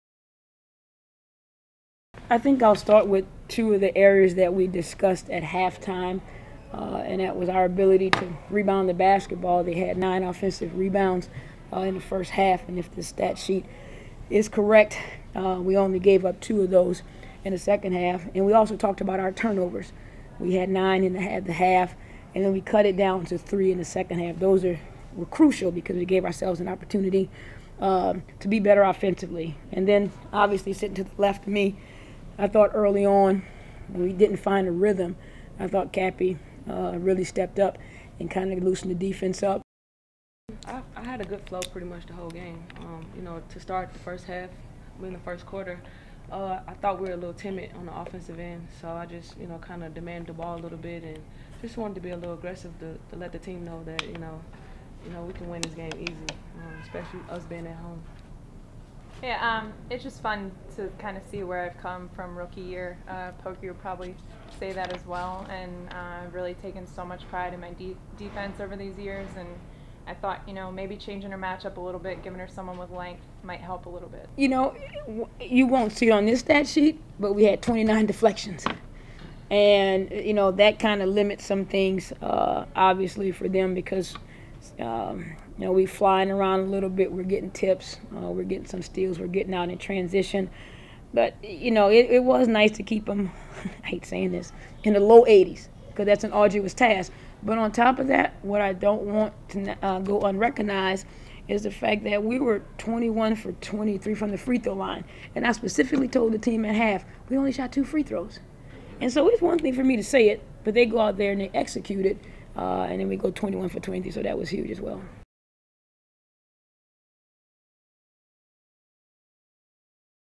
Post Game Presser: Sky vs. Liberty 07/10/15
The Chicago Sky (7-5) returned to Allstate Arena on Friday night to take on the Minnesota Lynx (8-3) in the first contest of a three-game home stand. Much to the delight of the crowd of nearly 7,000 fans, the Sky defeated the Lynx 90-83 behind 24 points from Cappie Pondexter and a 24-point, 10 rebound double-double from Elena Delle Donne.